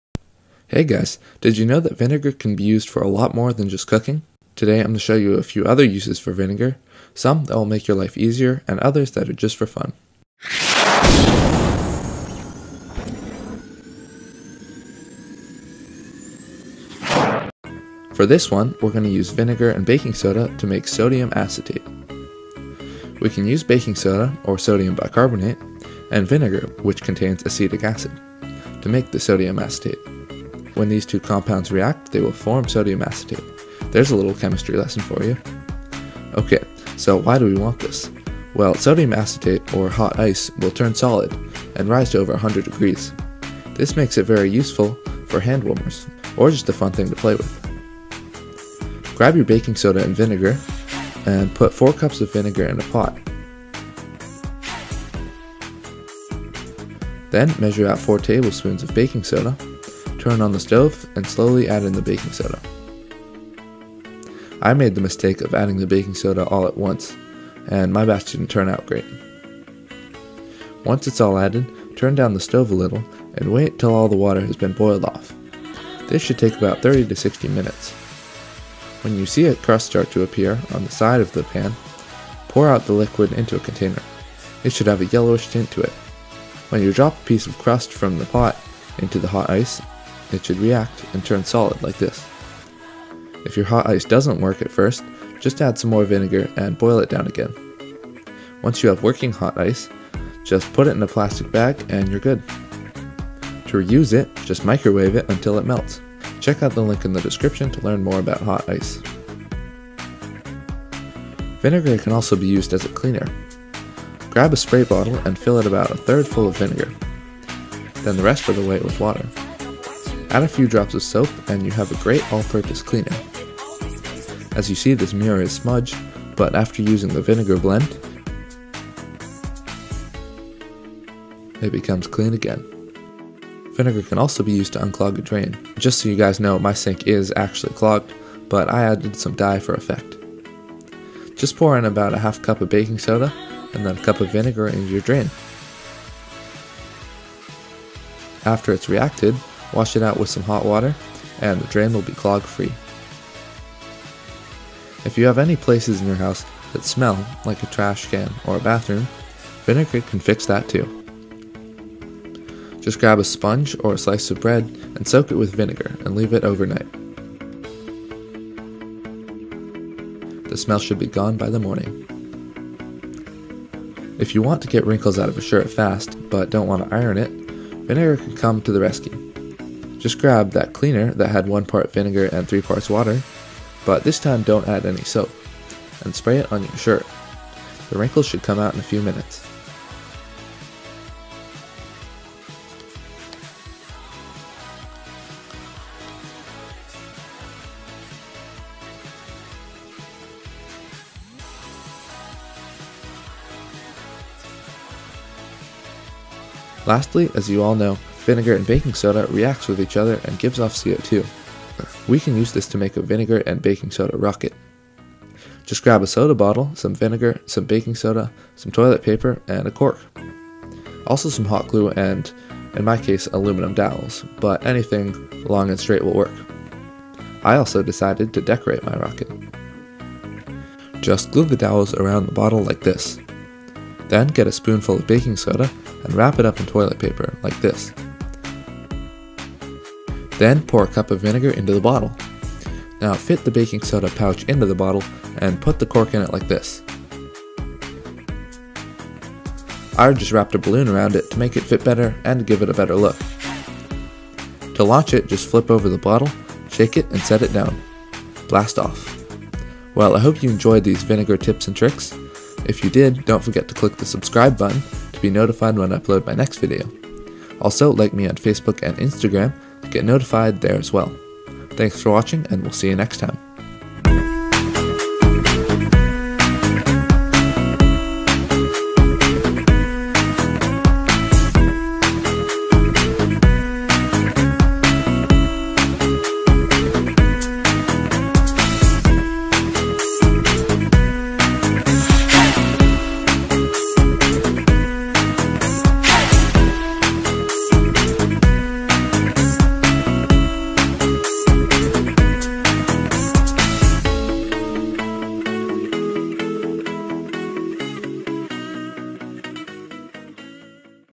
6 Awesome Life Hacks! - Vinegar Edition
Did you know that vinegar can be used for more than just cooking? This video is on 6 tips and tricks to do with vinegar.
#1 Make hot ice using vinegar and backing soda. Hot ice or Sodium Acetate has properties that allow it to turn solid and raise to over 100º when activated. This makes it a cool thing to play with, and great for some home made hand warmers.
#2 Vinegar can also be used as a cleaner. Just at fill a bottle 1/3 with vinegar and the rest with water. Then just ad a little bit of soap, and your done.
#3 Vinegar can also be used to unclog a drain. Just pore 1/2 cup of baking soda and 1 cup of vinegar into the drain. Then wash it with hot water, your drain should be un clogged.
#4 Vinegar can get rid of smells as well. Just grab a sponge or a slice of great and soak it with vinegar. Just leave it in a smelly spot over night, and the smell should be better by the morning.
#5 To get wrinkles out of a shirt fast, just mix 1 part vinegar to 3 parts water. Spray it on a shirt and in a few minutes the wrinkles will be gone.
#6 Lastly as you all know vinegar and backing soda reacts with each other and give off CO2. We can use this to make a vinegar and backing soda rocket. Just grab a soda bottle, some vinegar, some backing soda, some toilet paper, and a cork. Also some hot glue and dowels. Just glue the dowels around the bottle. Then get a spoonful of backing soda and wrap it up in toilet paper like this. Then pore in a cup of vinegar. Now fit the backing soda pouch into the bottle and put the cork in it like this. To launch it, just flip over the bottle, shake it, and set it down. Blast off.